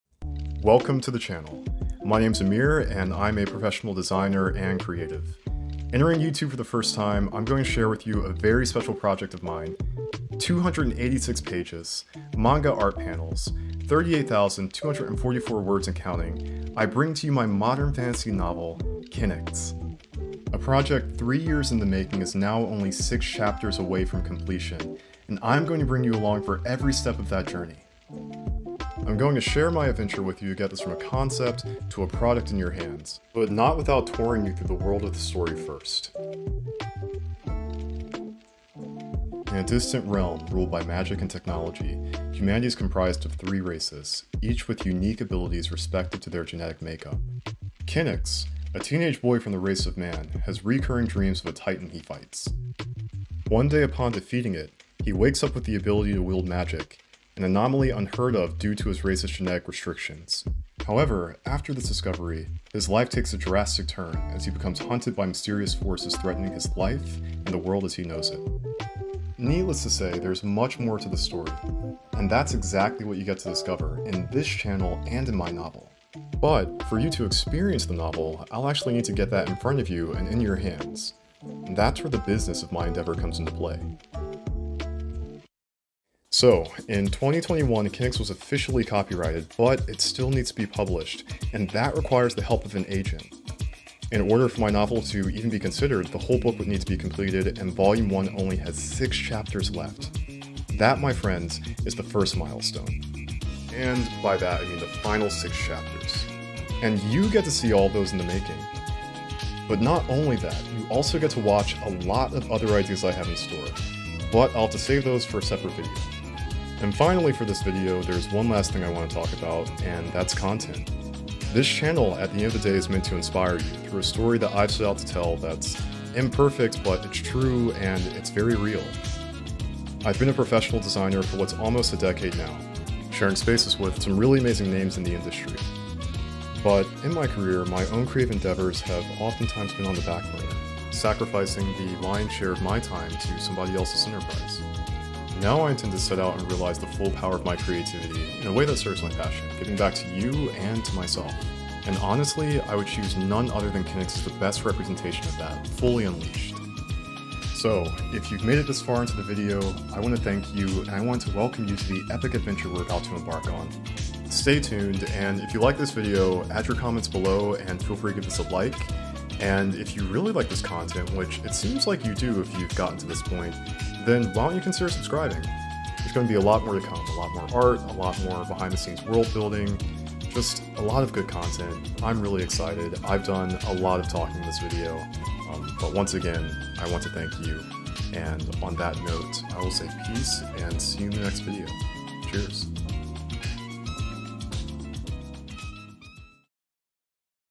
The official welcome to CREATIVE ANIMUS
A very warm welcome to creatives and viewers watching. Today I'm showing you the window into the world of my imagination with amazing story told through Creative Animus.